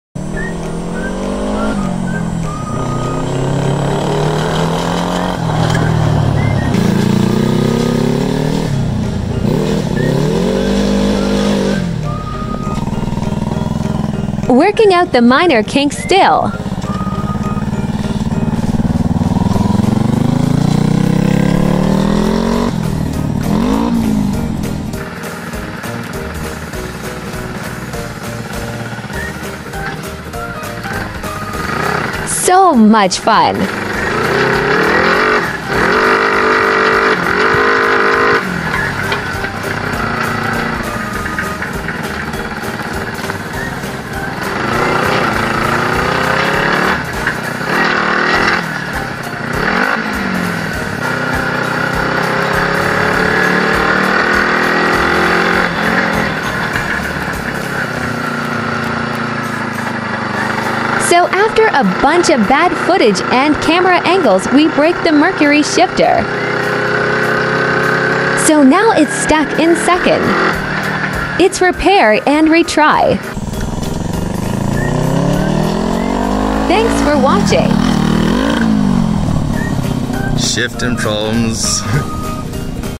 Driving the Worlds First Off-Road Reverse Ninja Trike!
We Test Drive the Ninja trike till We break it!